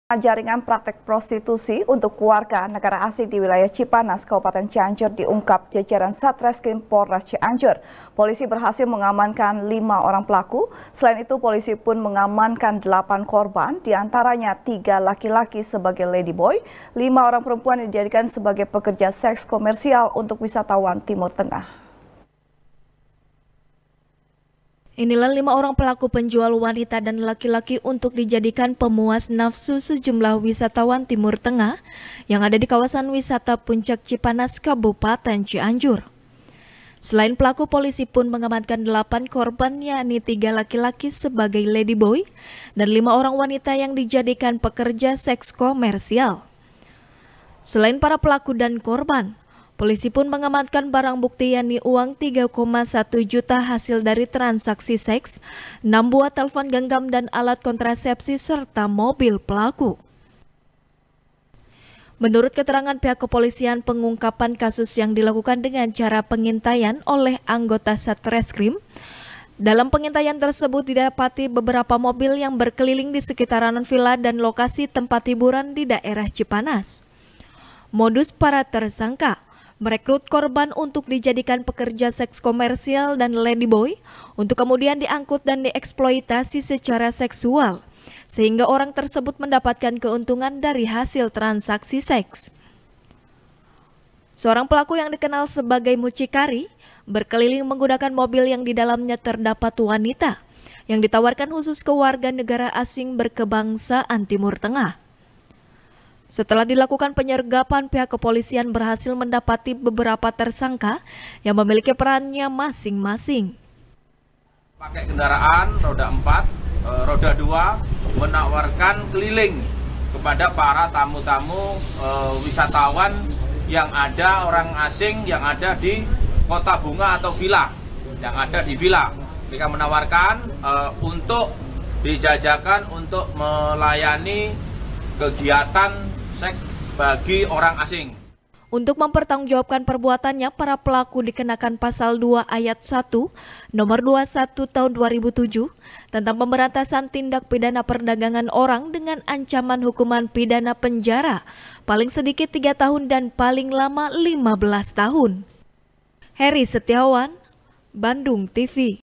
Ditangkap, Lima Pelaku Jaringan Prostitusi Trafficking | SBR | BANDUNGTV
SBR | BANDUNGTV | FARHAT MUMTAZ | Jaringan Praktek Prostitusi Untuk Warga Negara Asing Di Wilayah Cipanas, Kabupaten Cianjur, Diungkap Jajaran Satreskrim Polres Cianjur. Polisi Berhasil Mengamankan Lima Orang Pelaku. Selain Itu, Polisi Pun Mengamankan Delapan Korban, Diantaranya Tiga LakiLaki Sebagai Ladyboy, Lima Orang Perempuan Yang Dijadikan Sebagai Pekerja Seks Komersial, Untuk Wisatawan Timur Tengah.